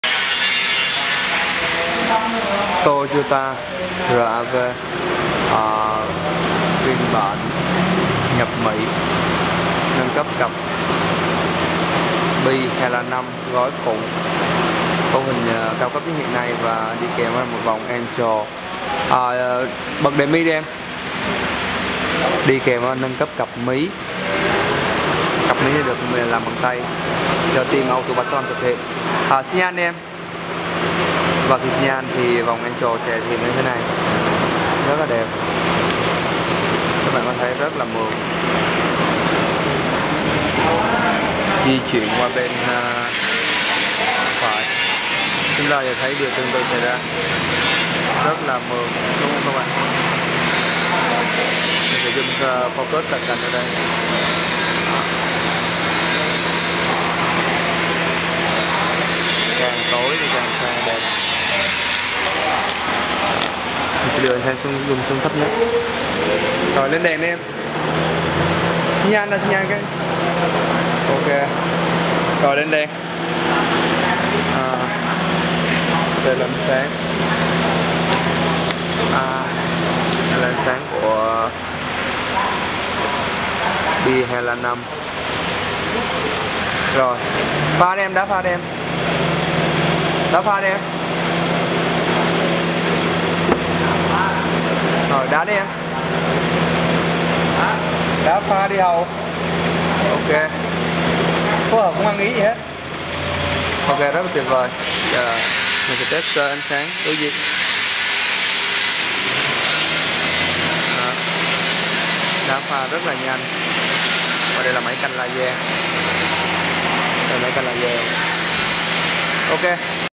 Toyota RAV Nhâp Khẩu từ Mỹ Độ Gói Đèn Khủng - Auto365
Độ cặp bi Hella5 gói khủng + vòng angel + mí led
AUTO365.VN - ĐỈNH CAO CÔNG NGHỆ
Add: 4/4/1/7 Đường số 3 Khu Hiệp Bình Phước - Q.Thủ Đức(Quốc lộ 13 gần Huyndai Ngọc An)